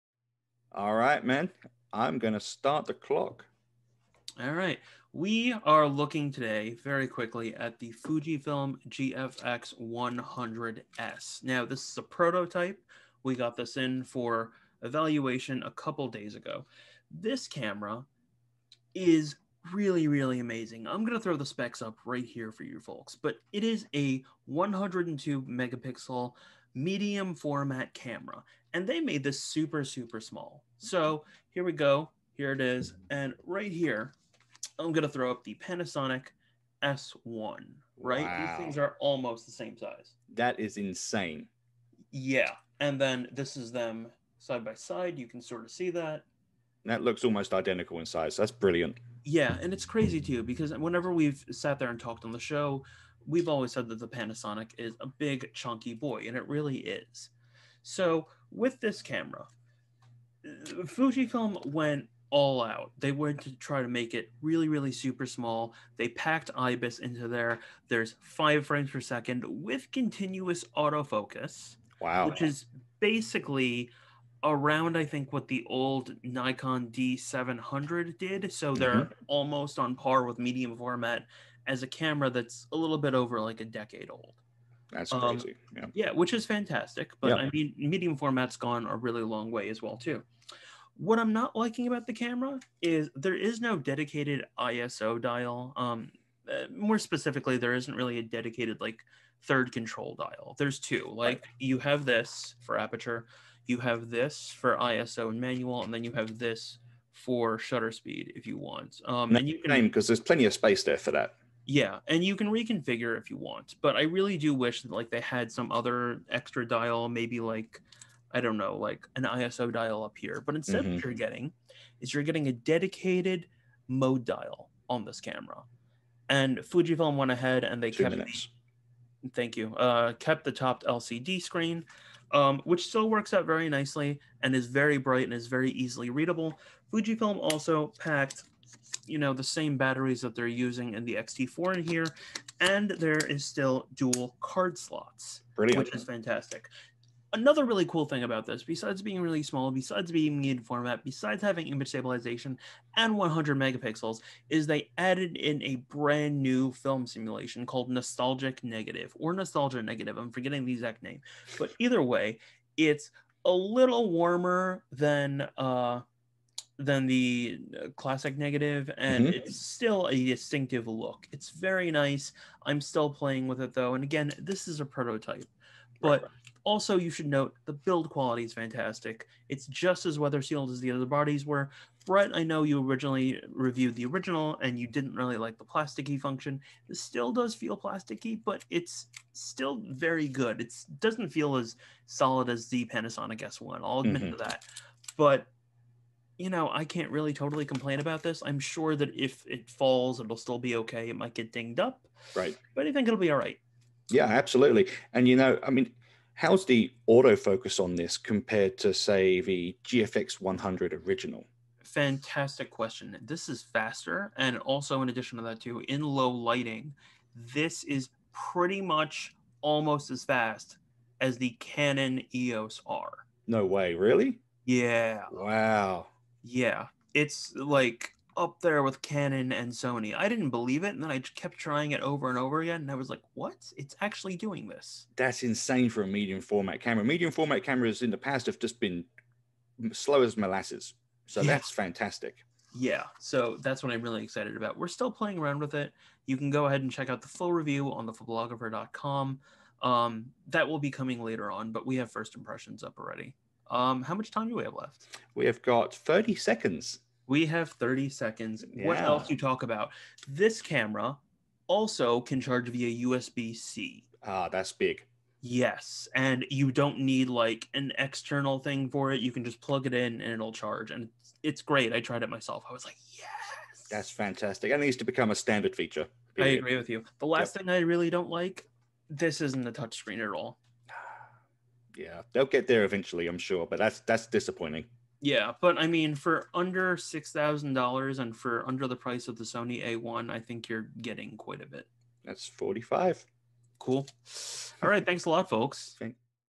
5 Min Camera Preview: Fujifilm GFX 100s
102 megapixels 43.8×32.9mm CMOS Sensor
X processor 4 (4:2:0 10bit / 12bit ProRes Raw 4K/30p)
5.0fps Continuous Shooting with AF-C / 64Gb DRAM
New Smaller Shutter & IBIS in smaller & Lighter weight body
Up to 6.0 stops Sync. Lens/IBIS / 4K30p non-crop recording
100% Phase Detection AF up to -5.5EV @ F1.7/ F13~16
All of the features from X-T4 and GFX100
DIS for Movie / ProRes Raw 12bit Output
400 Megapixel Multiple Shot, Drone & Gimbal support
GFX S (shooting) style Body H150 x V104 x D44mm
New Battery NP-W235 in grip makes the body thinner
Weather and Dust resistant, Freeze Proof to -10°C
Initial Shipment: Feb/Mar 2021
Launch Price: US $5,999